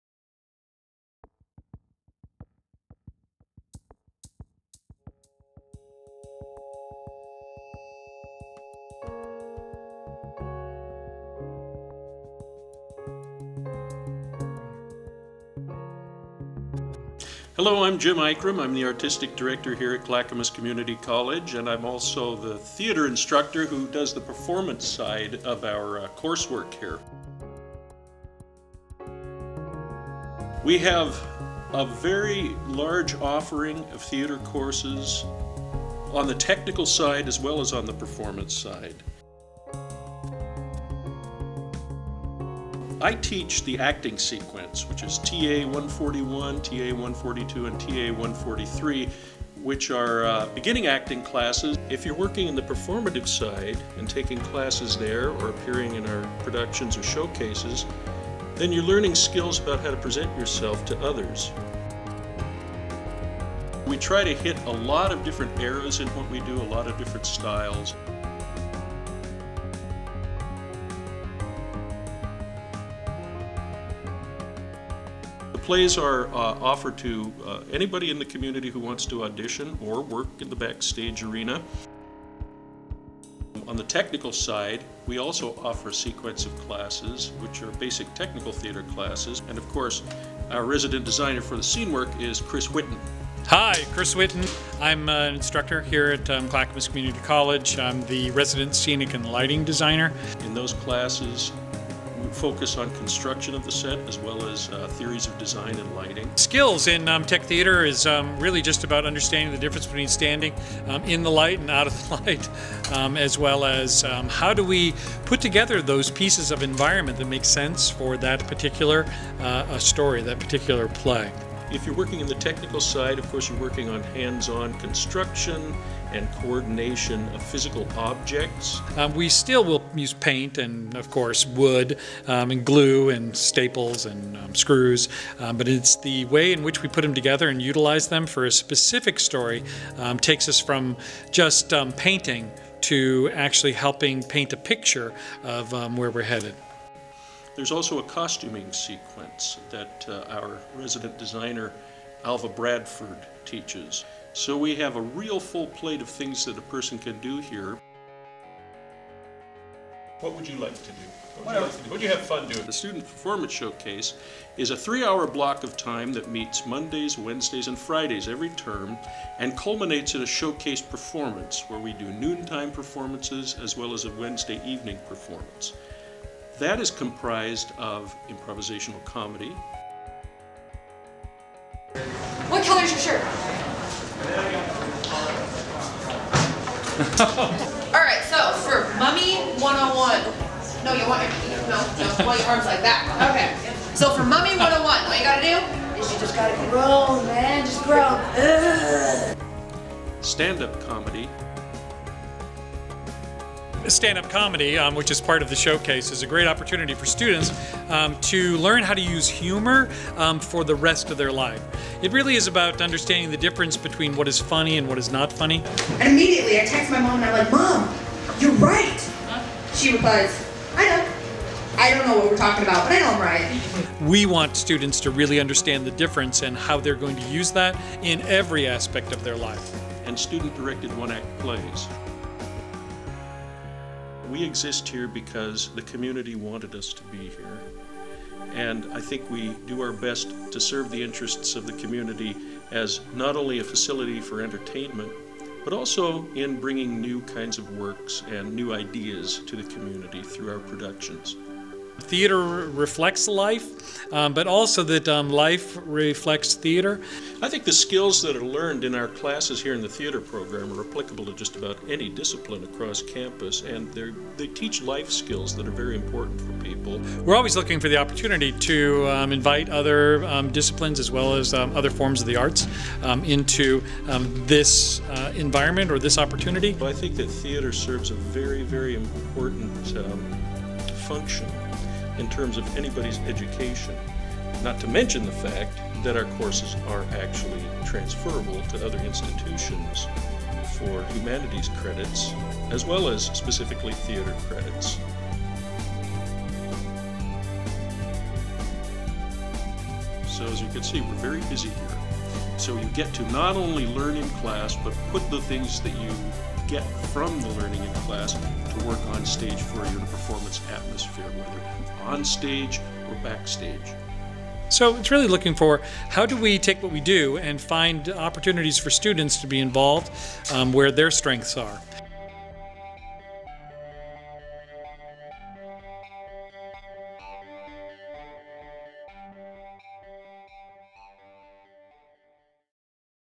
Clackamas CC Theater Arts Program
The Theater Arts program at Clackamas Community College offers classes in acting, directing, theater production and set design.  for more information.  Digital story told by Jennifer Whitten - a final project for DMC 104 Digital Video Editing.